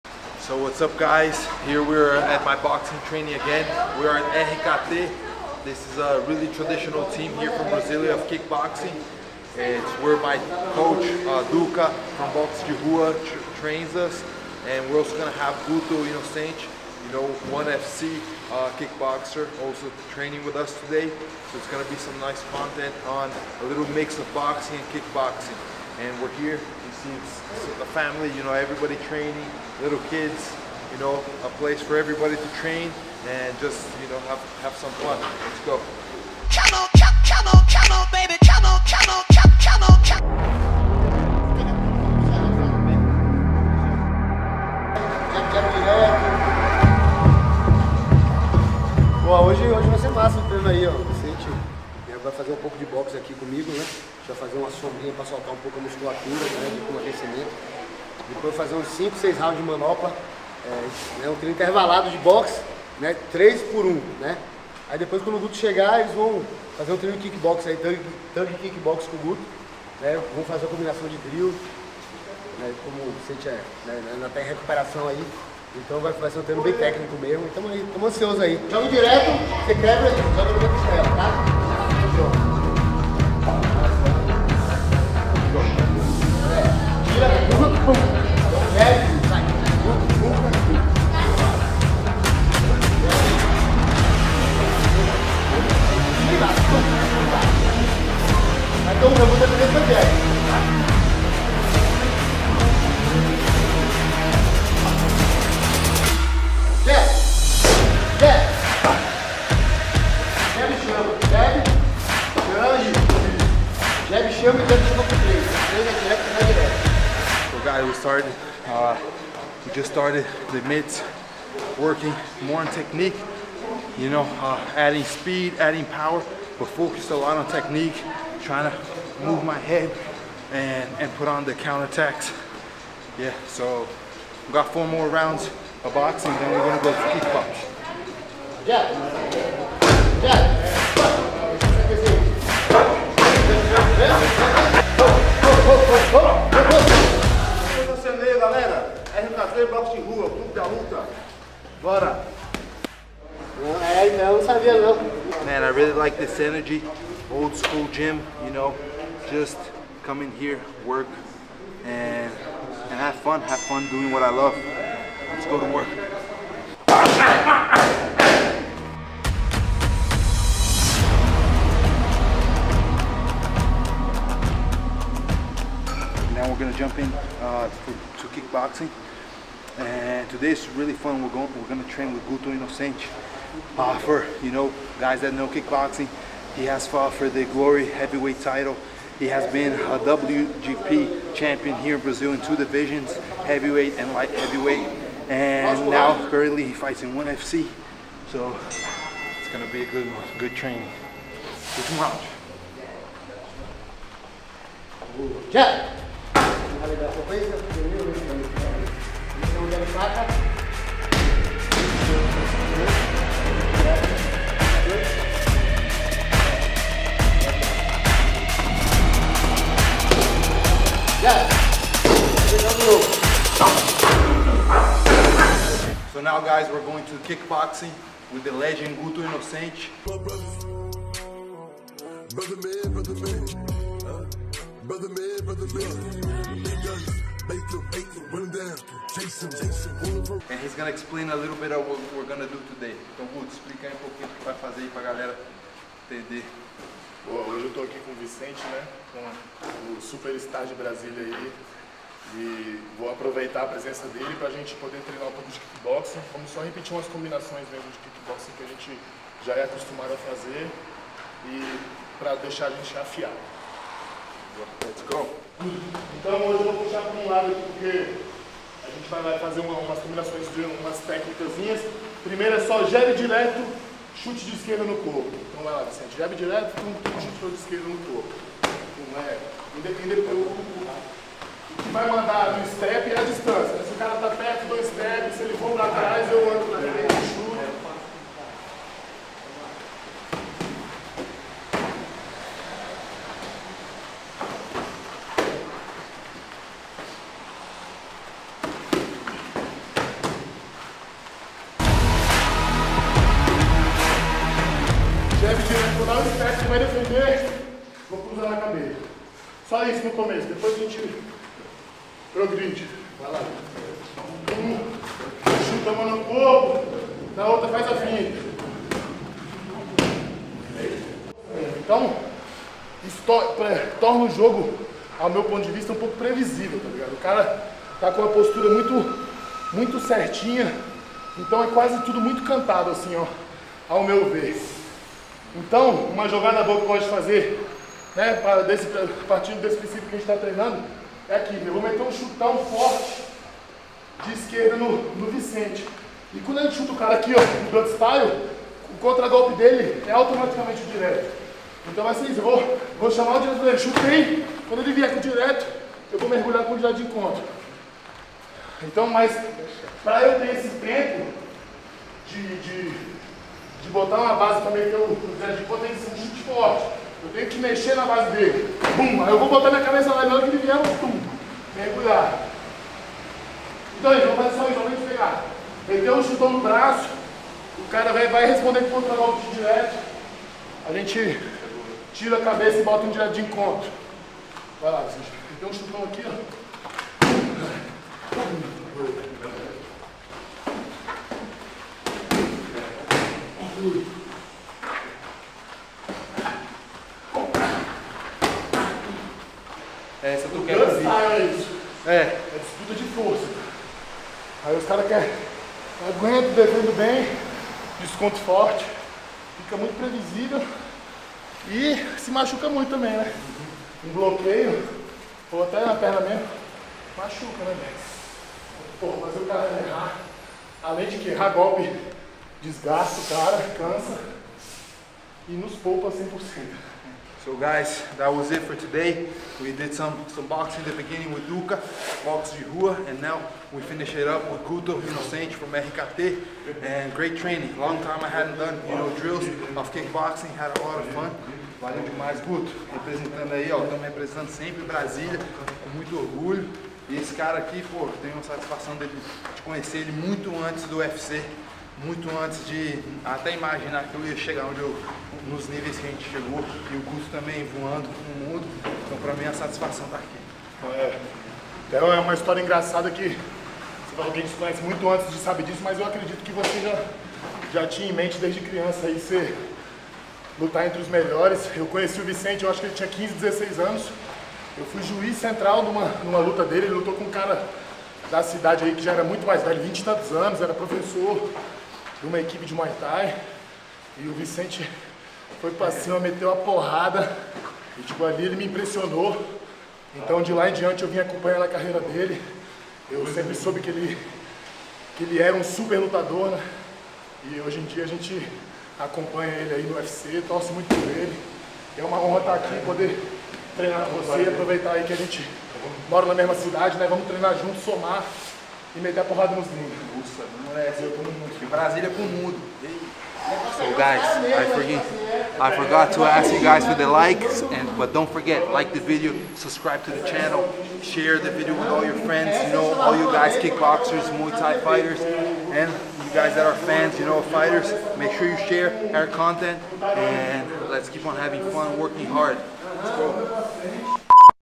Boxing work and Kickboxing drills with Guto Inocente!
Some great boxing work with Duk from Boxe de Rua and finished of the practice doing some drills with this Kickboxing beast Guto Inocente (@gutoinocente) who is also from my hometown of Brasilia.

Videomaker @hugomoreirafilms